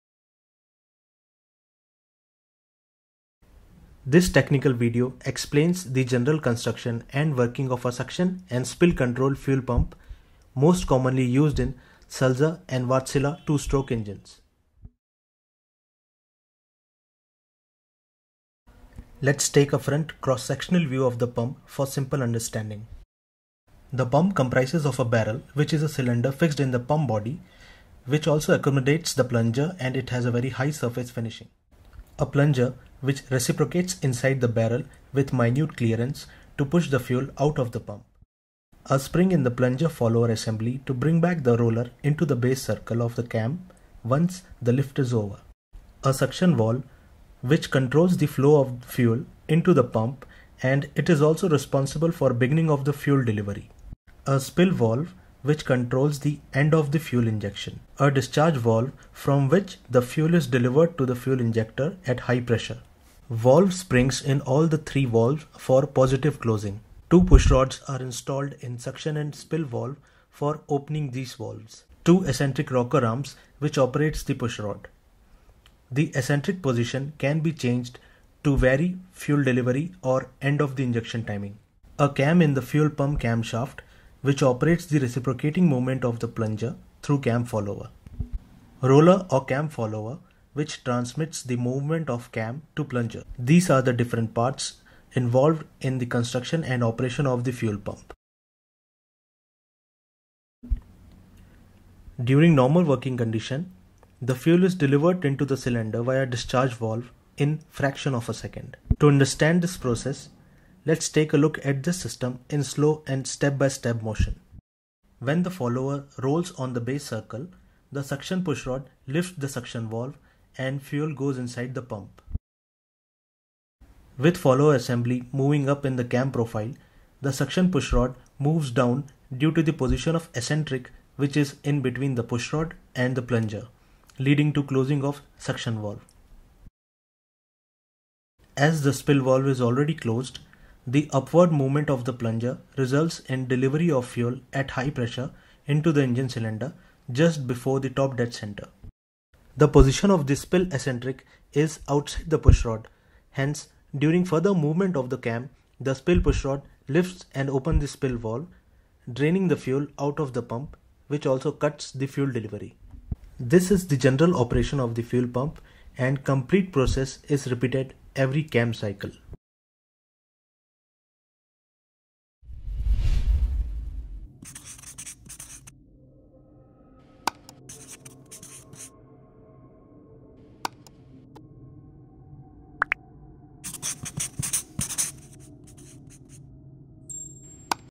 Main Engine Fuel Pump Working Explained
The main engine fuel pump is complex machinery fitted with several mechanical parts which ensure that the engine cylinders get the necessary fuel at high pressure with required atomisation. One of the most common types of marine engine fuel pump used on the ship’s engine is the spill and suction valve controlled fuel pump.

It is important for marine engineers working on ships to understand the basics of these fuel pumps to carry out troubleshooting, repairs and make necessary setting when required in the correct manner.

This main engine fuel pump working video tutorial explains the basics of the marine fuel pump i.e. its construction and working.